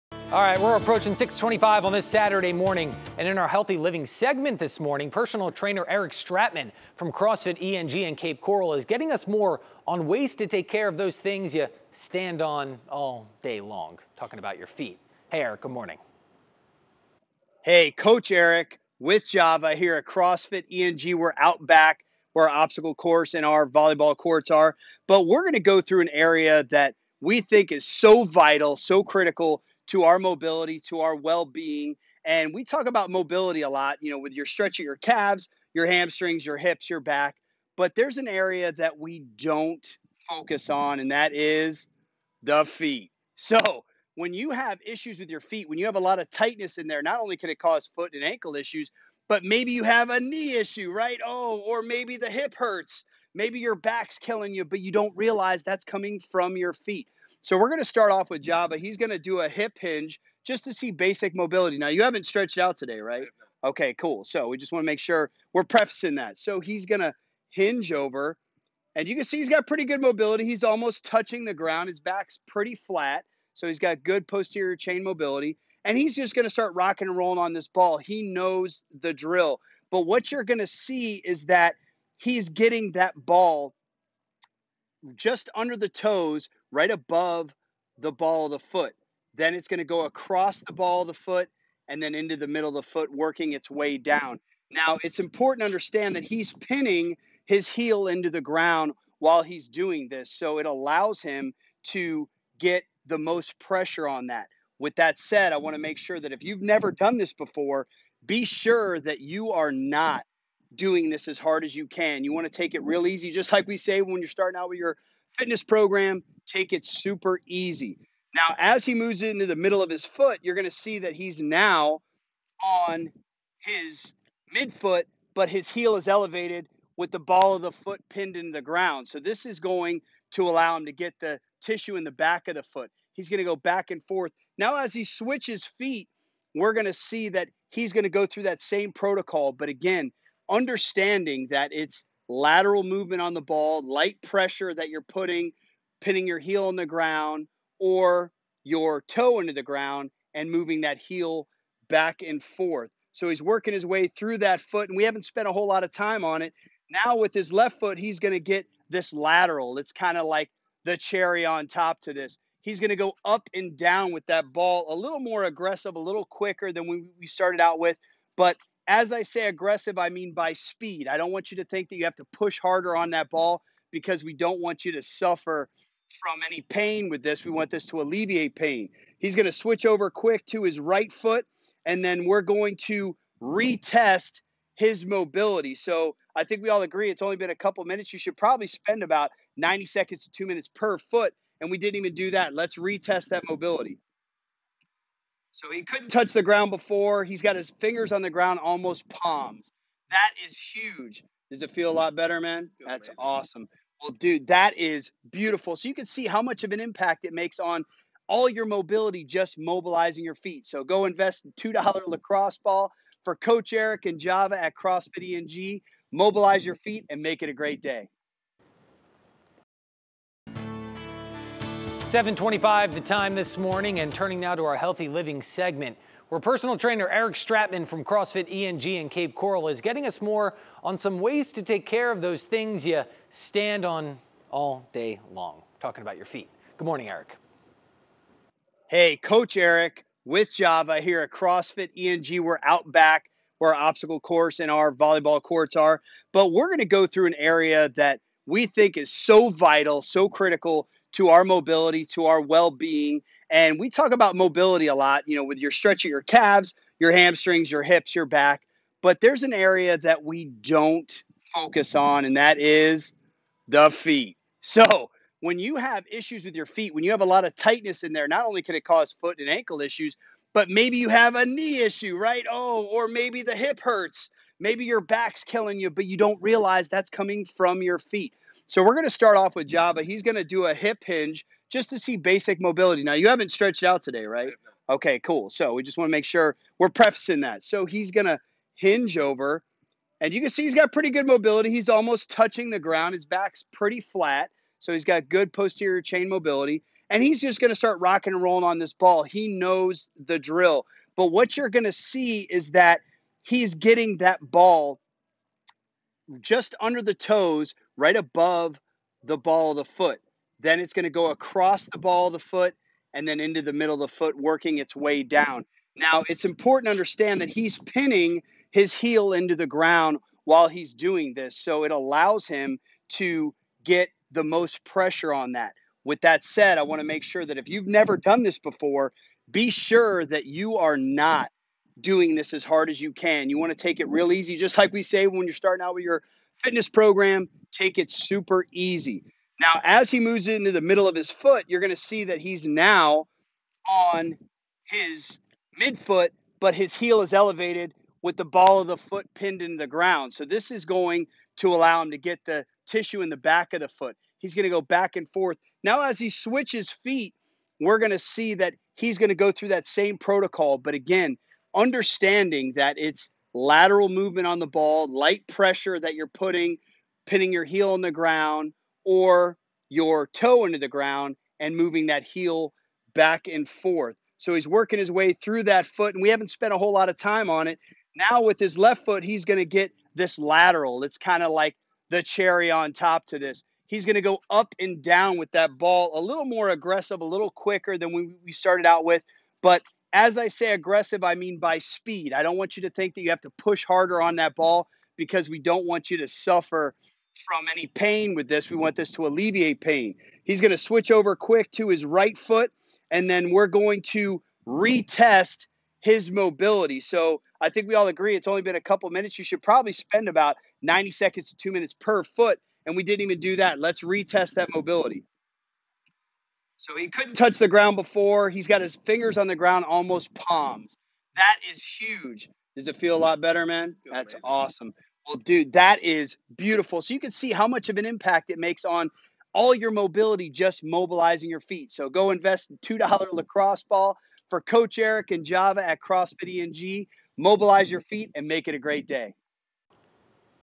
FOOTCARE w/ Coach Eric ABC7 GUEST SEGMENT
Take care of your feet and they will take care of you. Here is how to start mobilizing them for whole-body health.